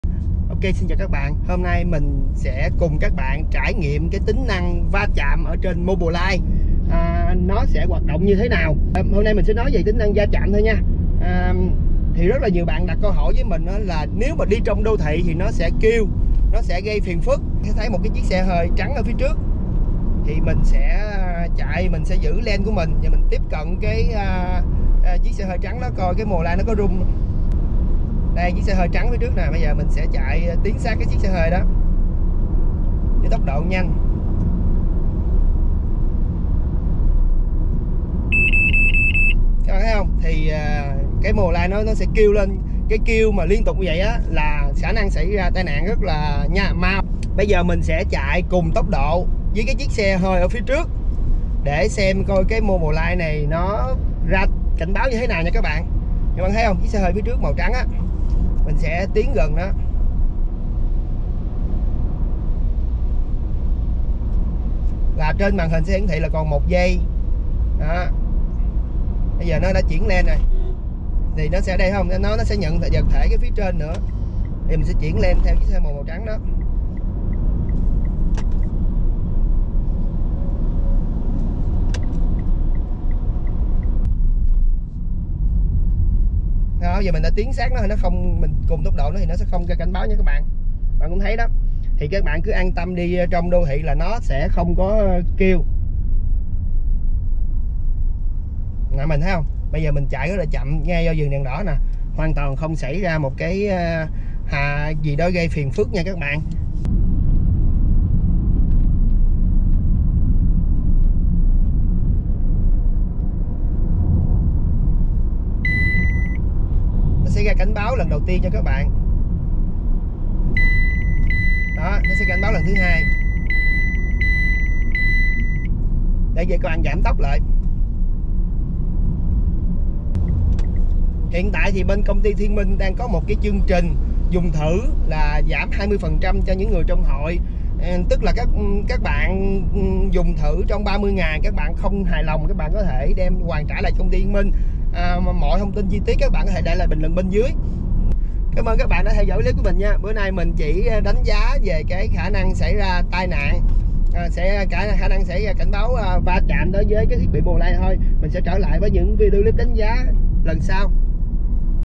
Xpander Test khả năng xảy ra va chạm trên Mobileye
Test khả năng xảy ra va chạm trên Mobileye
Nếu bạn nào chạy giỏi rồi thì xem vui thôi đừng phán tội nghiệp em, chứ em là quen với cảnh đem xe đi sửa rồi ... còn đợi đến người chắc 8 người đem em đi